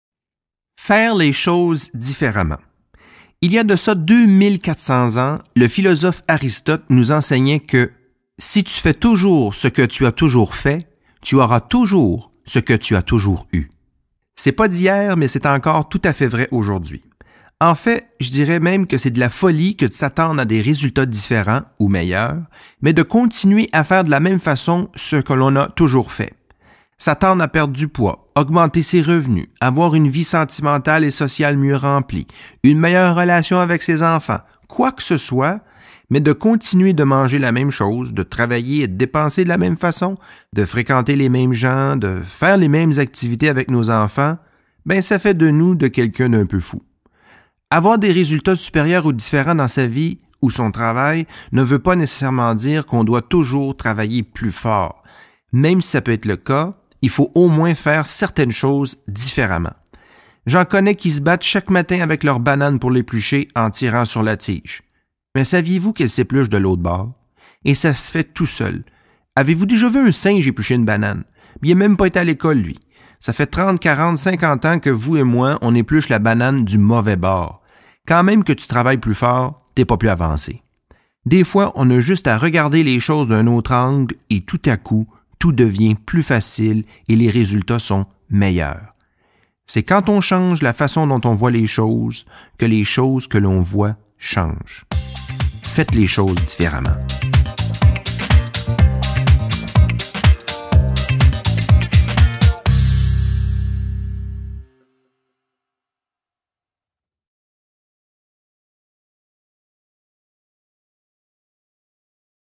Faire les choses différemment | Marc André Morel
Lue par le conférencier en entreprise et auteur à succès Marc André Morel, cette courte capsule audio vous inspire et enseigne sur l'importance de faire les choses différemment pour faire du chemin sur vos projets et réaliser vos rêves.

Contactez-nous!